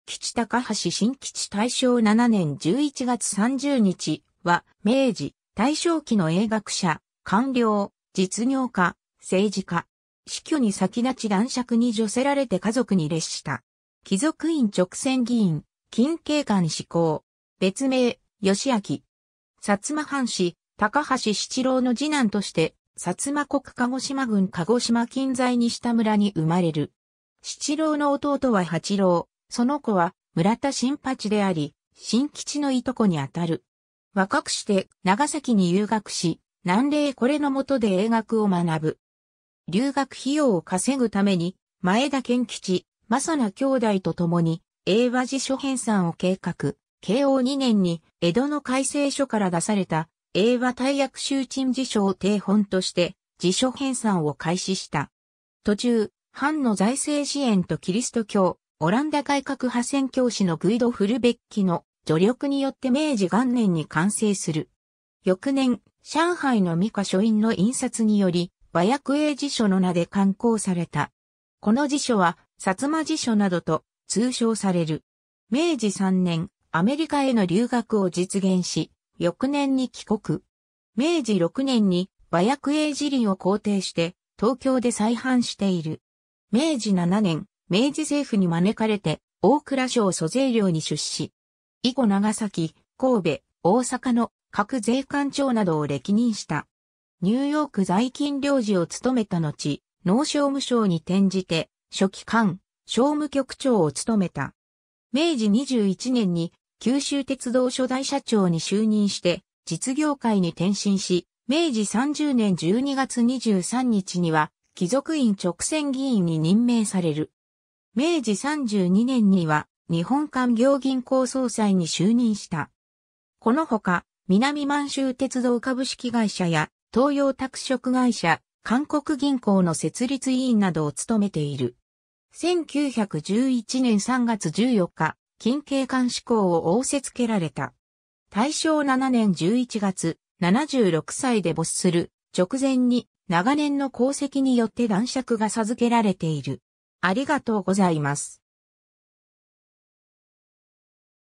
高橋新吉 (英学者)
高橋新吉 高橋 新吉（たかはし しんきち、天保14年1月28日（1843年2月26日）- 大正7年（1918年）11月30日）は、明治・大正期の英学者・官僚・実業家・政治家。
死去に先立ち男爵に叙せられて華族に列した。
貴族院勅選議員、錦鶏間祗候。
別名・良昭（よしあき）。
 薩摩藩士高橋七郎（良顕）の次男として薩摩国鹿児島郡鹿児島近在西田村（現在の鹿児島市常盤）に生まれる。
七郎の弟は八郎、その子は村田新八であり、新吉の従兄にあたる。
 若くして長崎に遊学し、何礼之の許で英学を学ぶ。
留学費用を稼ぐために前田献吉・正名兄弟とともに英和辞書編纂を計画、慶応2年（1866年）に江戸の開成所から出された「英和対訳袖珍辞書」を底本として辞書編纂を開始した。
途中、藩の財政支援とキリスト教オランダ改革派宣教師のグイド・フルベッキの助力によって明治元年（1868年）に完成する。
翌年、上海の美華書院（米国長老派教会系の出版社）の印刷により『和訳英辞書』の名で刊行された。
この辞書は、「薩摩辞書」などと通称される。
明治3年（1870年）、アメリカへの留学を実現し、翌年に帰国。
明治6年（1873年）に『和訳英辞林』を校訂して東京で再版している。
 明治7年（1874年）、明治政府に招かれて大蔵省租税寮に出仕。
以後長崎・神戸・大阪の各税関長などを歴任した。
ニューヨーク在勤領事を務めたのち、農商務省に転じて書記官・商務局長を務めた。
明治21年（1888年）に九州鉄道初代社長に就任して実業界に転進し、明治30年（1897年）12月23日には貴族院勅選議員に任命される。
明治32年（1899年）には日本勧業銀行総裁に就任した。
このほか、南満州鉄道株式会社や東洋拓殖会社、韓国銀行の設立委員などを務めている。
1911年3月14日、錦鶏間祗候を仰せ付けられた。
 大正7年（1918年）11月、76歳で没する直前に、長年の功績によって男爵が授けられている。